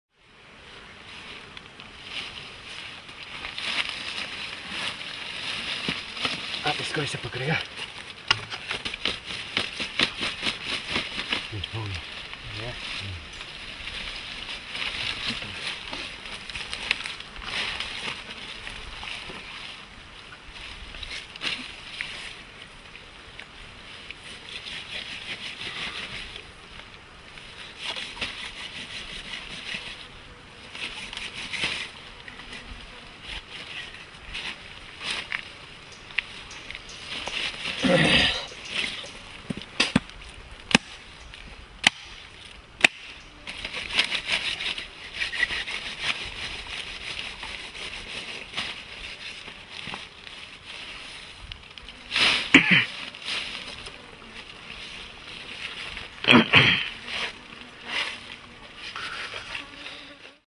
Hacking a wild boar
Butchering a wild boar. Game meat being cut up.
Wild boar (Sus scrofa), also known as wild pig, is a species of the pig genus Sus, part of the biological family Suidae. The species includes many subspecies. It is the wild ancestor of the domestic pig, an animal with which it freely hybridises.[3] Wild boar are native across much of Northern and Central Europe, the Mediterranean Region (including North Africa's Atlas Mountains) and much of Asia, including Japan and as far south as Indonesia. Populations have also been artificially introduced in some parts of the world, most notably the Americas and Australasia. Elsewhere, populations have also become established after escapes of wild boar from captivity.[4]
Source: Wikipedia